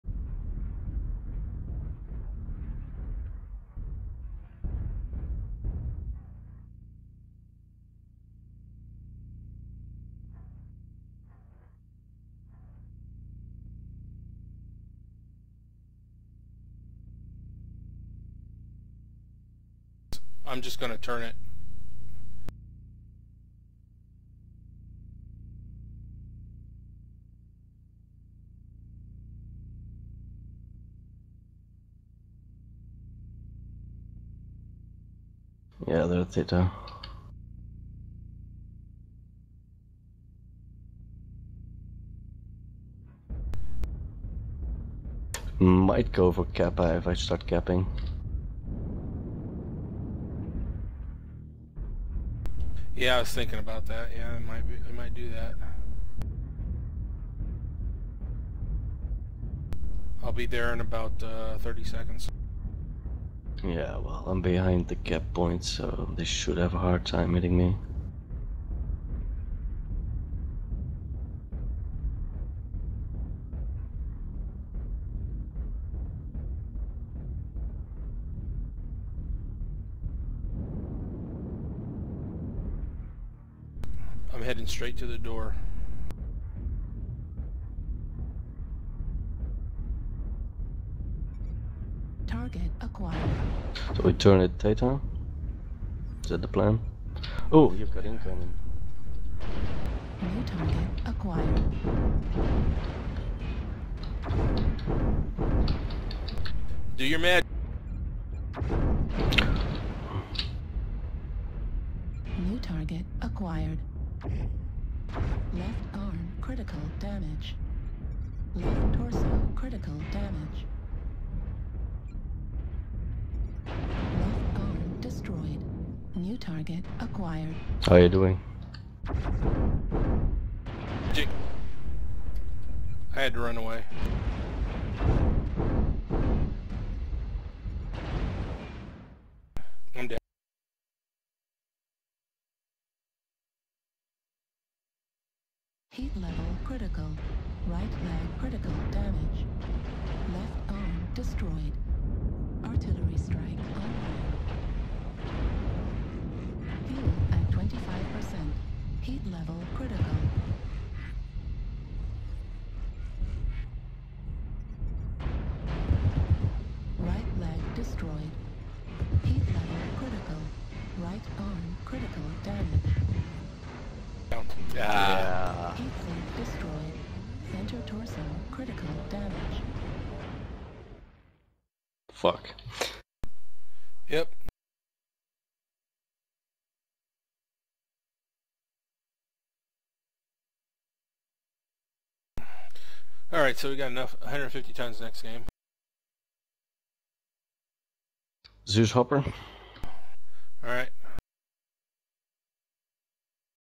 BSI 2v2 Knuckleheads VS Geared Lightning 2
Blue Star Irregulars 2v2 Round Robin

Knuckleheads VS Geared Lightning

Knuckleheads: Grendel408 & Krystof82
Geared Lightning: Nostirius & Hopper Morrison